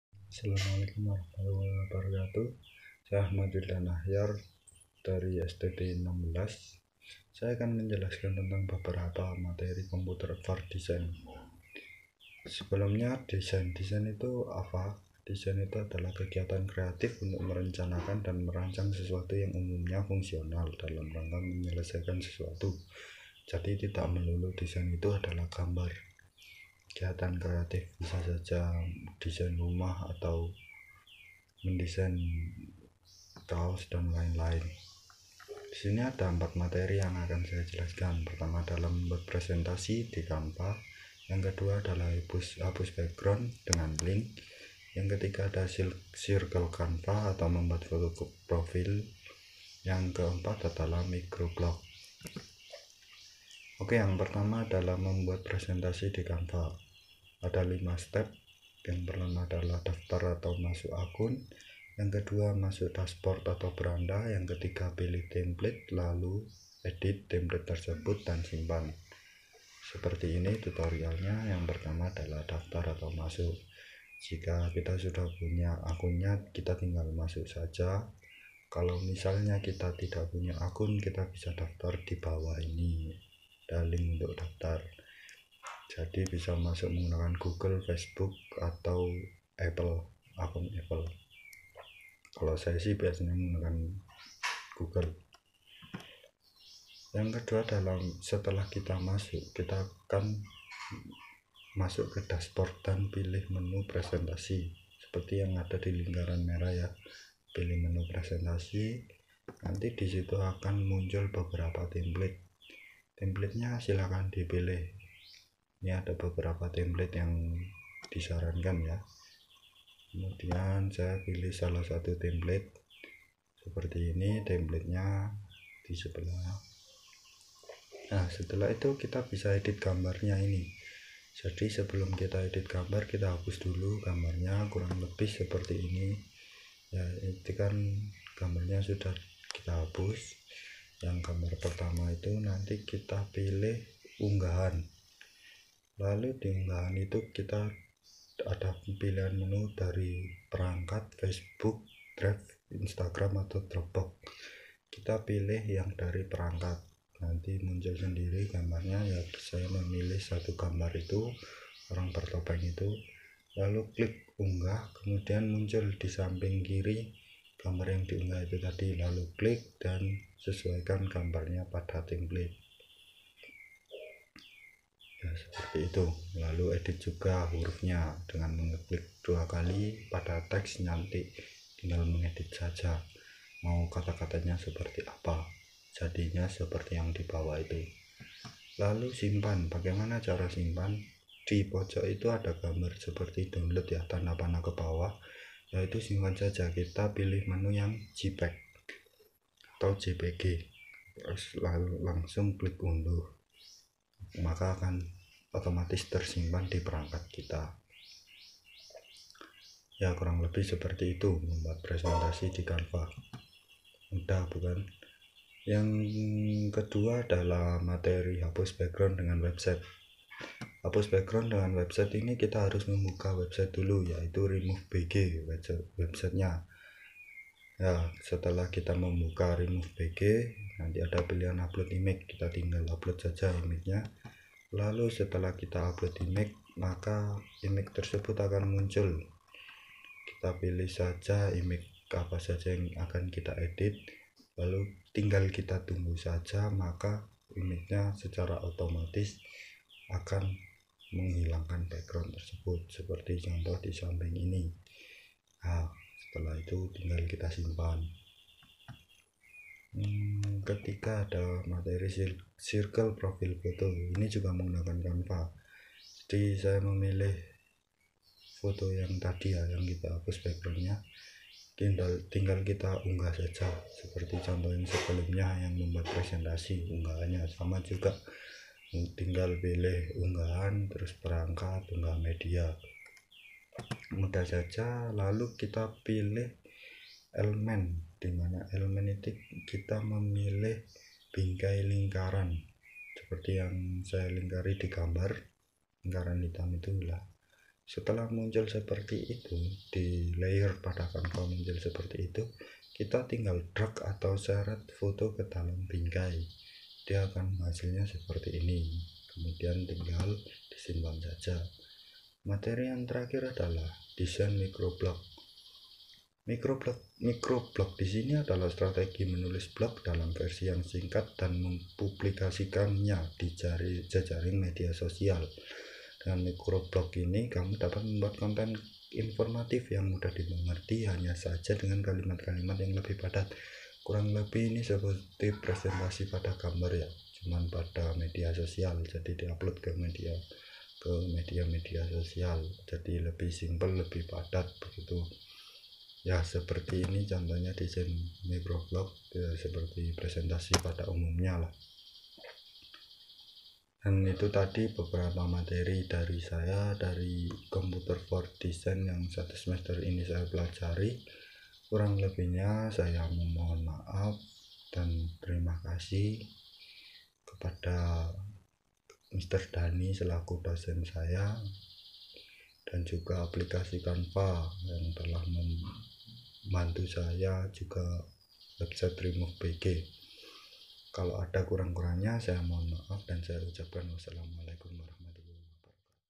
MATERI COMPUTER FOR DESIGN
saya Achmad Wildan Achyar Mahasiswa STT ingin membagikan dasar ilmu tentang computer for design.